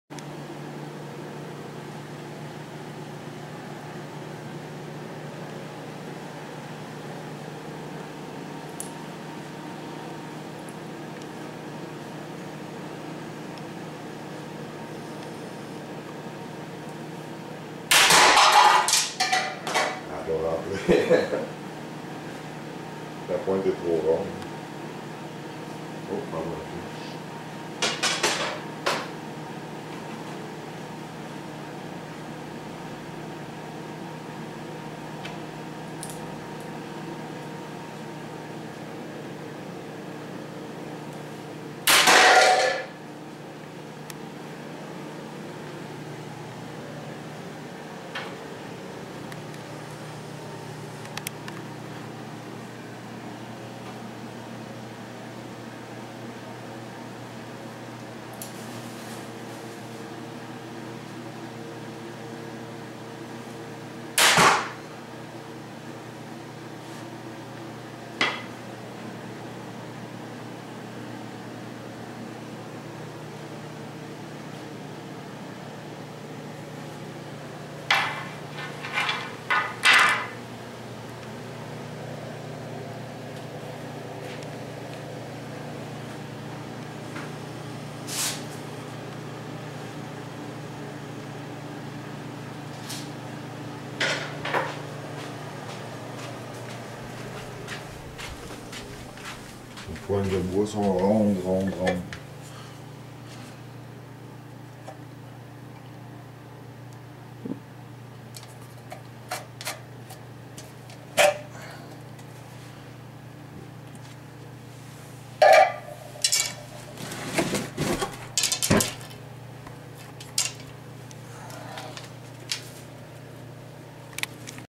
Arc en orme rouge, red elm hunting bow.
L'arc présenté est fait d'orme rouge du Nebraska. Arc d une puissance de 70 lbs a 28 pouce ou # 70 at 28 inch. L'arc a été entierement travaillé a la main, lime et couteau puis formé a la vapeur. Cependant je recommande de faire le courbage a sec avec un séchoir a décaper. Ce traitement est meilleur pour le bois et permet de remettre la corde dans un délais pas mal plus court. Arc vernis a l'uréthane (MINWAX) 4 couches.